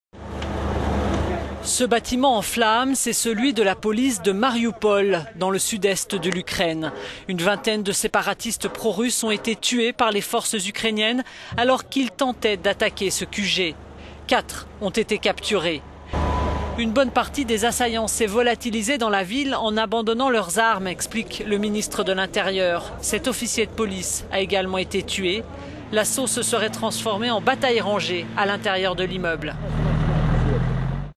Ukraine : bain de sang au siège de la police de Marioupol
Ce bâtiment en flamme, c'est celui de la police de Marioupol, dans le sud-est de l'Ukraine. Une vingtaine de séparatistes pro-russes ont été tués par les forces ukrainiennes alors qu'ils tentaient d'attaquer le QG. Quatre ont été capturés.  "Une bonne partie des assaillants s'est volatilisée dans la ville en abandonnant leurs armes, explique le ministre de l'Intérieur. Un officier de police a été tué.

L'assaut se serait transformé en bataille rangée à l'intérieur de l'immeuble.

== (fun...